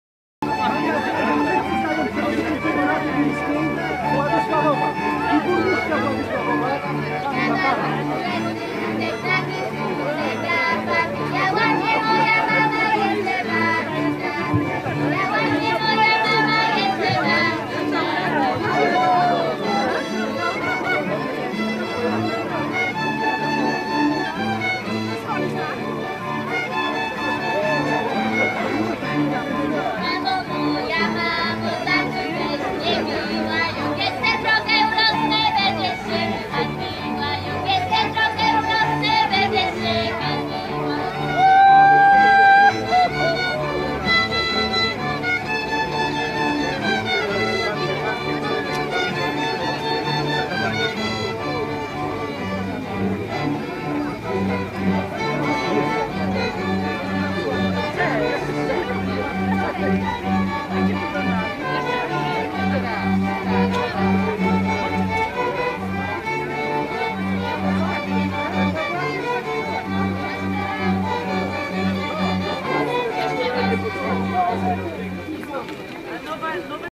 suski.DlaWas.info - Skrót: Dni Ziemi Makowskiej cz.1
skrót wiadomości z powiatu: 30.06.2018 r  Pierwszy dzień obchodów 640 rocznicy powstania Makowa Podhalańskiego